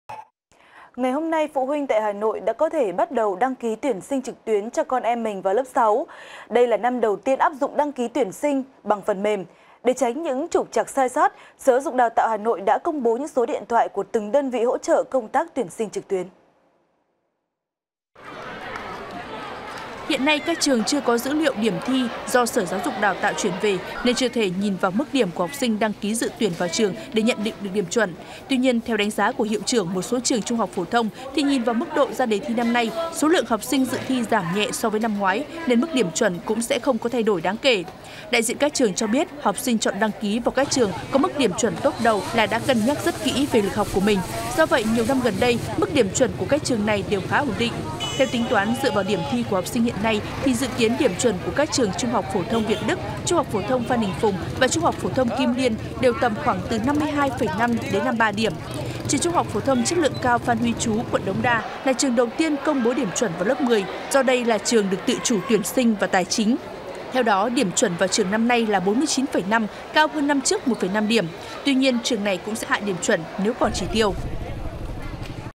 (VTC14)_Hà Nội: Kích hoạt tổng đài hỗ trợ tuyển sinh lớp 6
(VTC14) - Hôm nay, phụ huynh tại Hà Nội đã có thể bắt đầu đăng ký tuyển sinh trực tuyến cho con em mình vào lớp 6. Đây là năm đầu tiên áp dụng đăng ký tuyển sinh bằng phần mềm nên để tránh sai sót, Sở GD&ĐT Hà Nội đã công bố số điện thoại của từng đơn vị hỗ trợ công tác tuyển sinh trực tuyến.